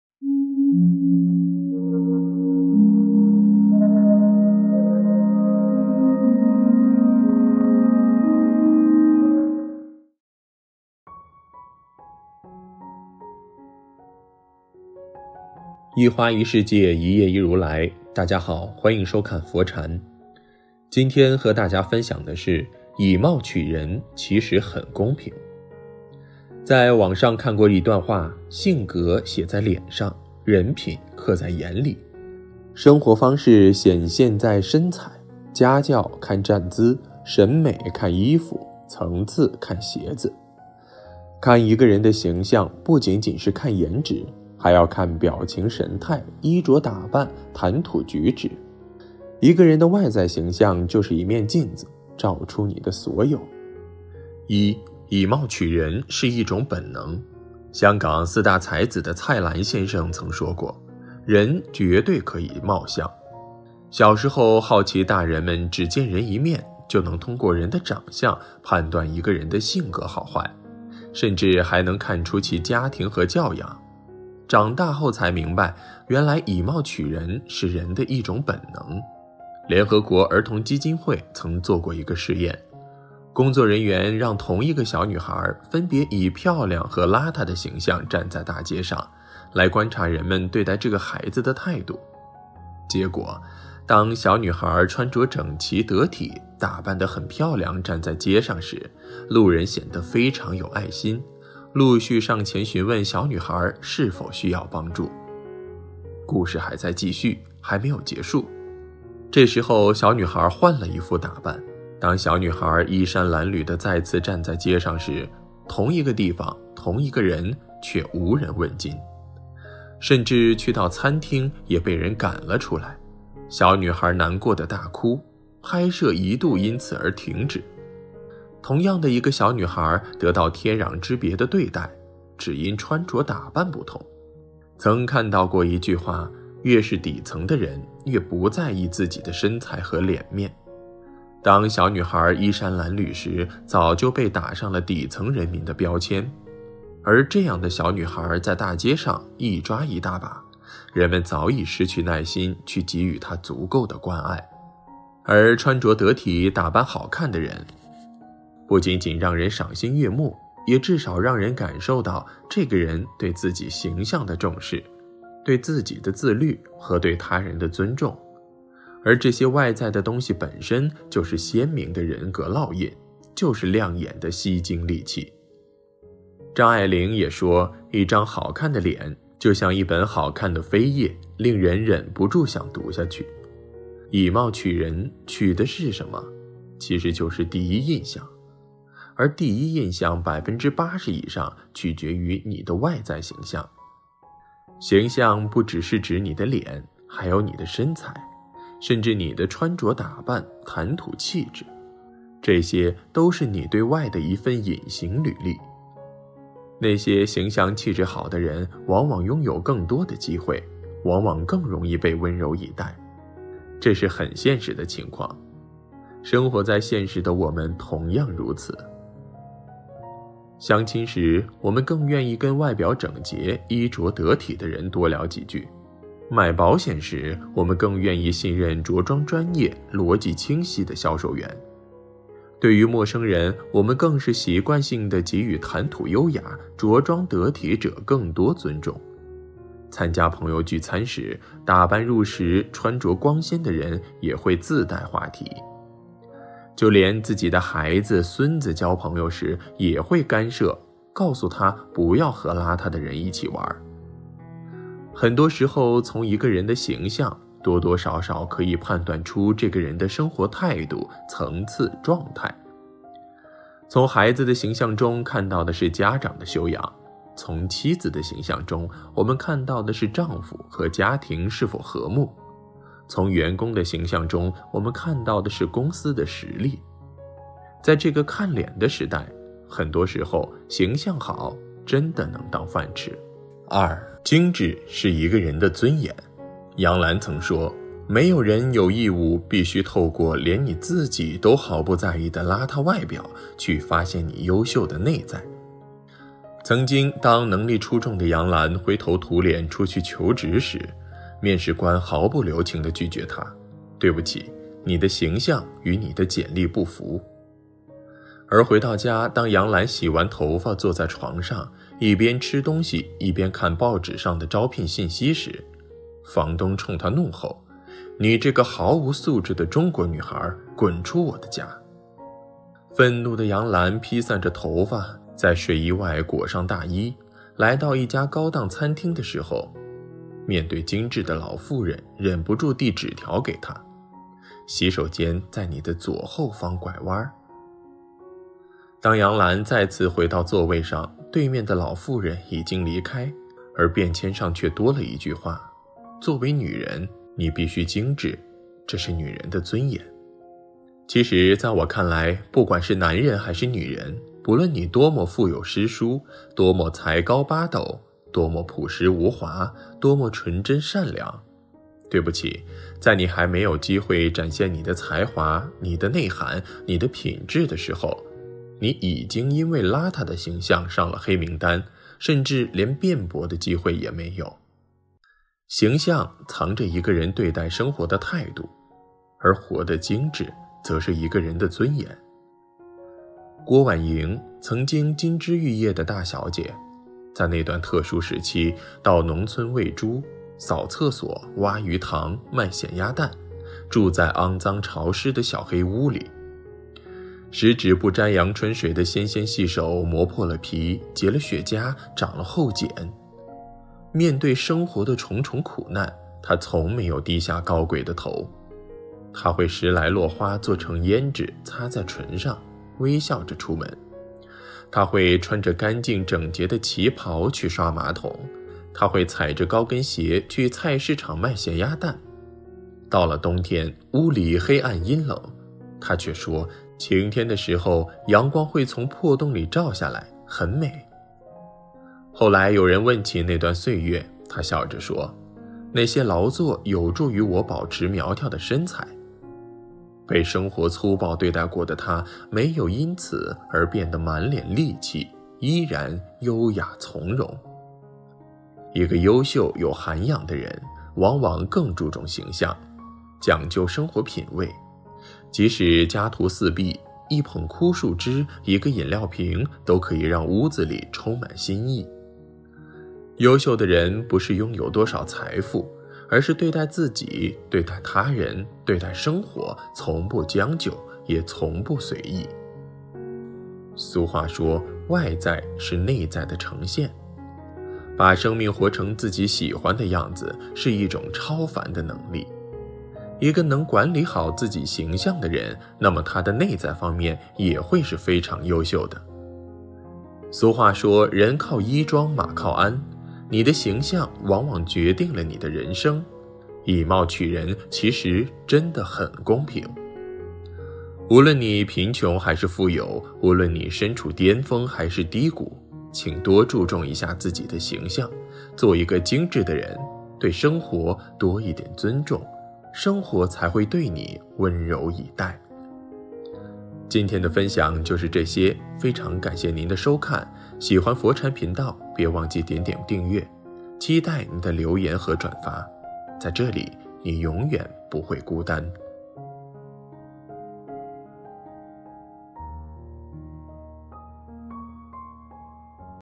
佛禪：「人可以貌相」，形象氣質好的人，往往擁有更多的機會
免責聲明：影片所用圖片源於網路，以上內容僅參考，如需解決具體問題，詳詢相關領域專業人士！